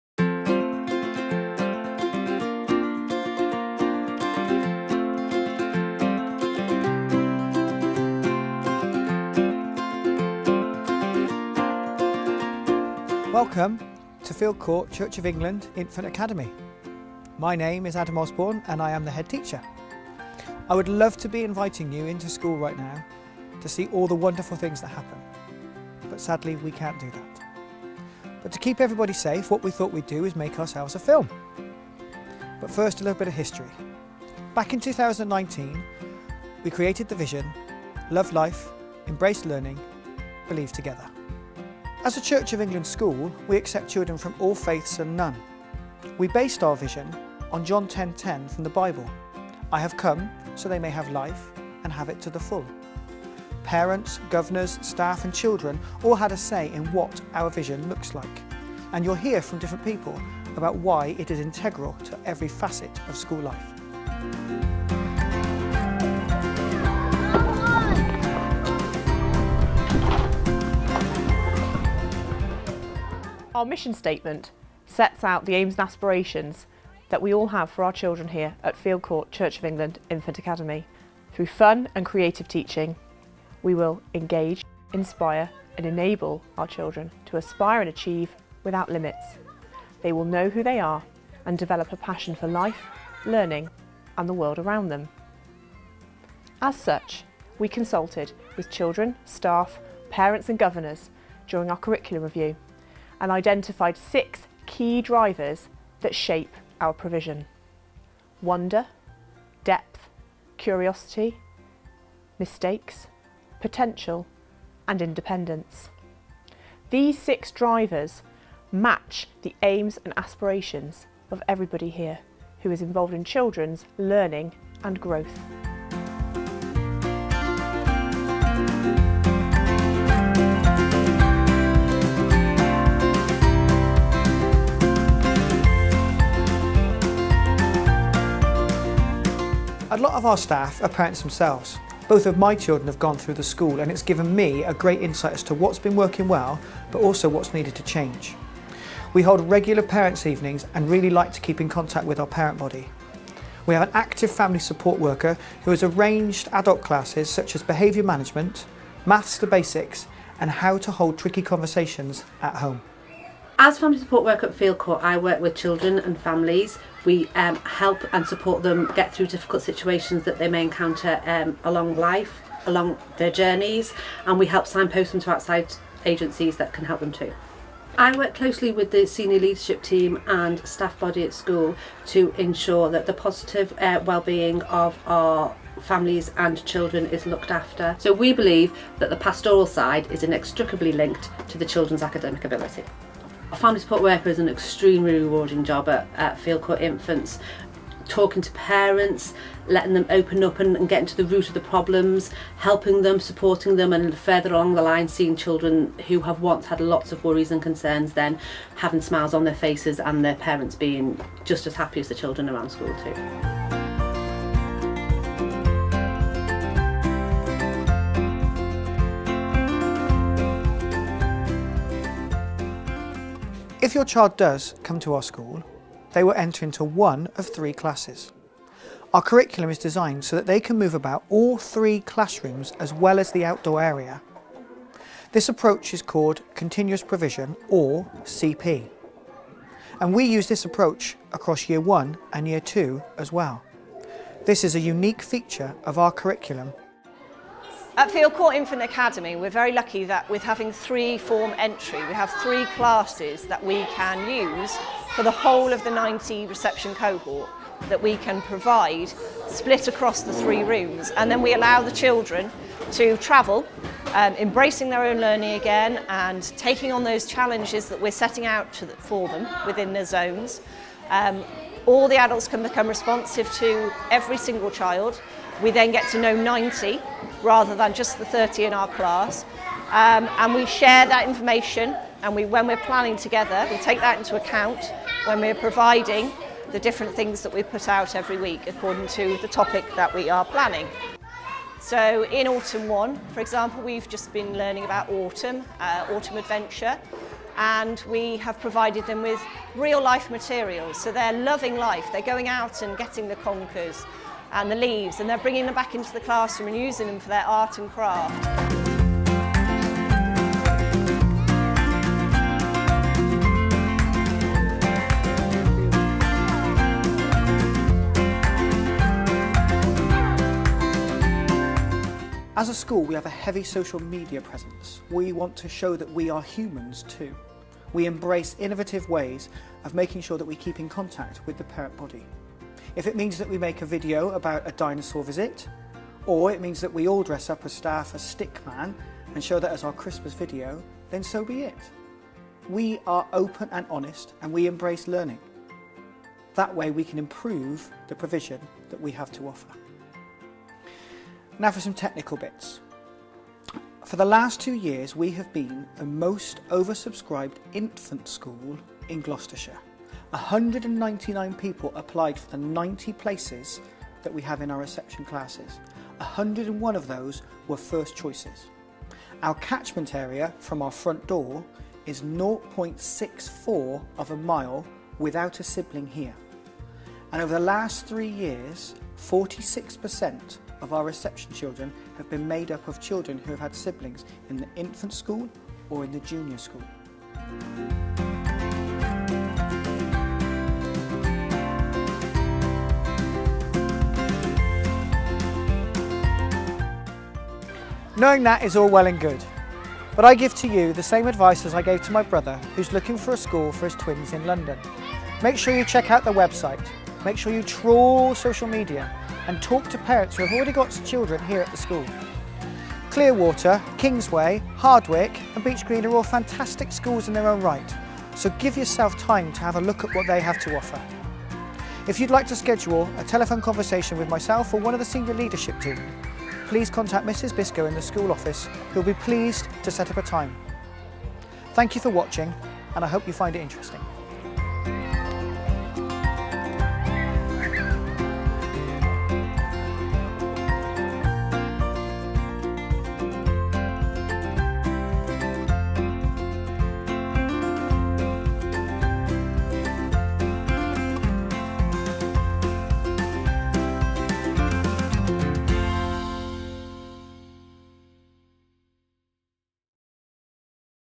Welcome to Field Court C of E Infant Academy for potential parents.
If you are looking for a Reception place in 2021, then this is the video for you.  It is a shame that we are unable to invite parents to tour our wonderful school, but we know you'll understand!

We would love you to view this video and think...I want to know more!  We know that our presence on social media has many other videos for you to view and although these are a great insight, they are not a question and answer session with myself or one of the team.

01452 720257

Thank you for your interest in our school and I hope to hear from you soon.

Kind regards,

Adam Osborne
Headteacher